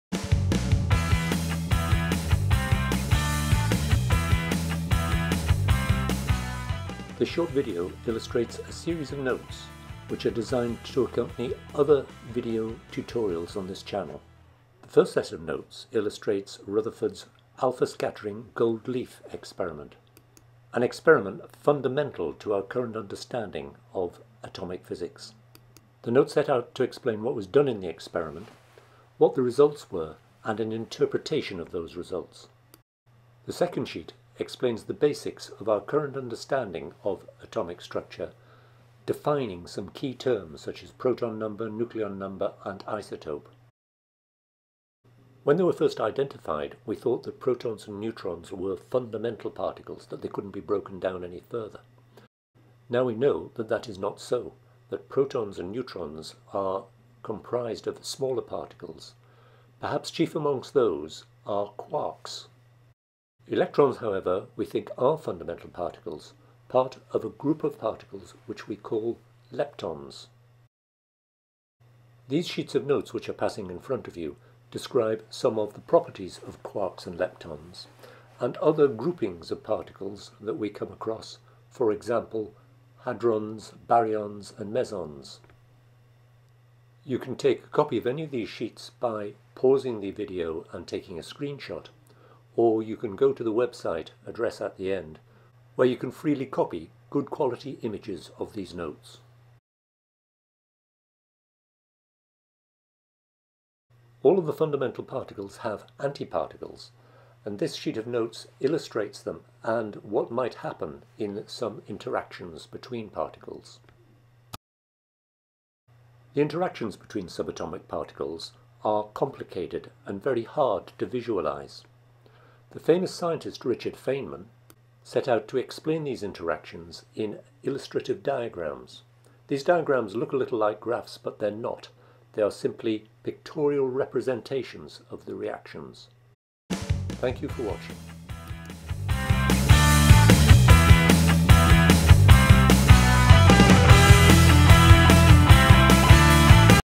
Notes on Sub-atomic particles - protons, neutrons, quarks and leptons: from fizzics.org
Notes on sub-atomic particles useful to students and connected to three video tutorials on this channel. The notes include basic atomic structure of nucleus, protons, neutrons and electrons. The further breakdown into (what we think are) fundamental particles of quarks and leptons and a little about interactions between particles and Feynman diagrams. The notes can be copied directly from the